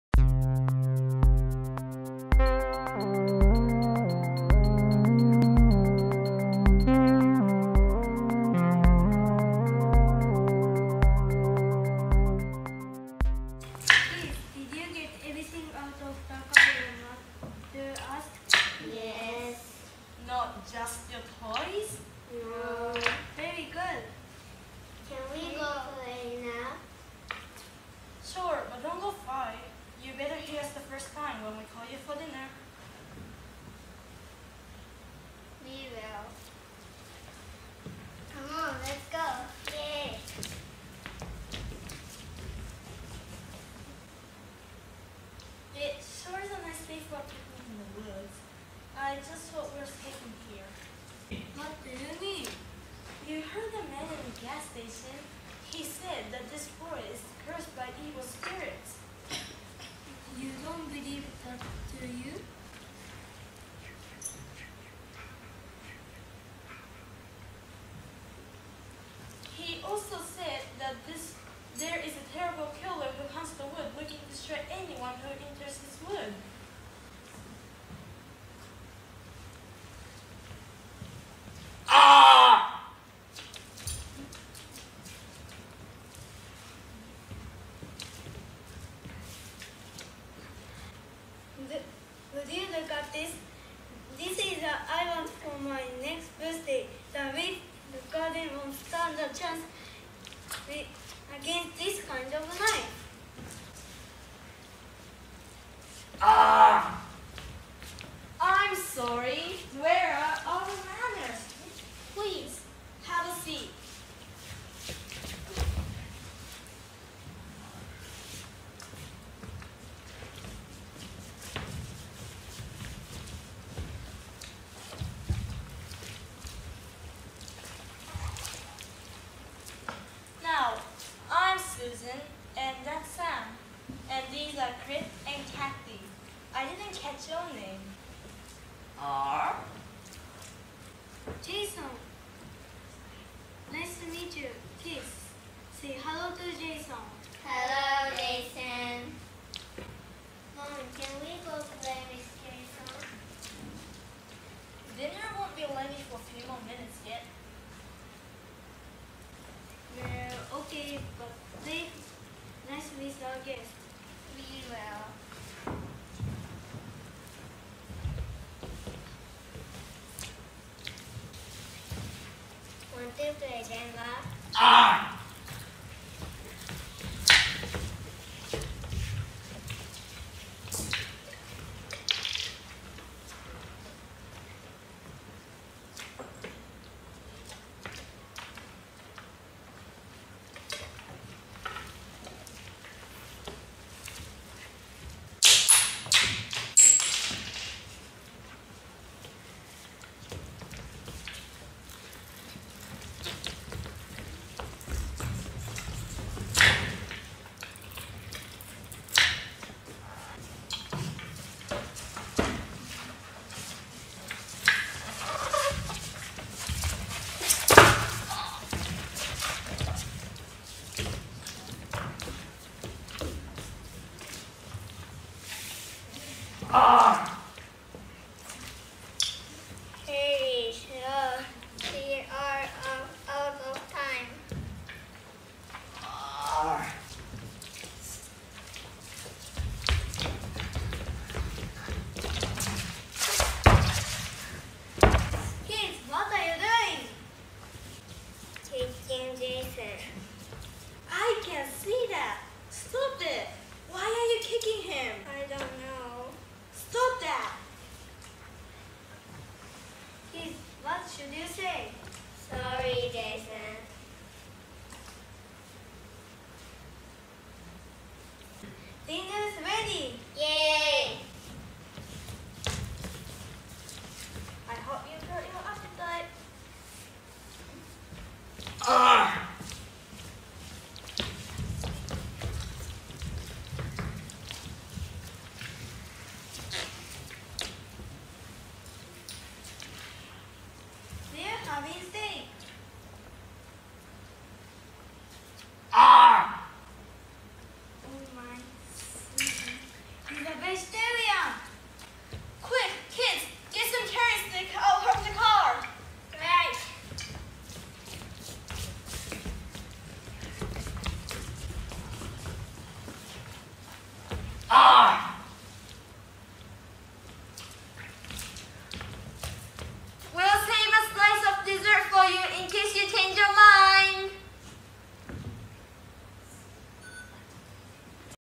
GKA Drama Club Picnic
The EDC (English Drama Club) celebrates the Halloween season with a silly scare experience we call ' The Picnic of Terror.'  Horror meets comedy as one clueless family is terrorized by a masked killer...or is it the other way around?  Enjoy this parody of camping horror tales.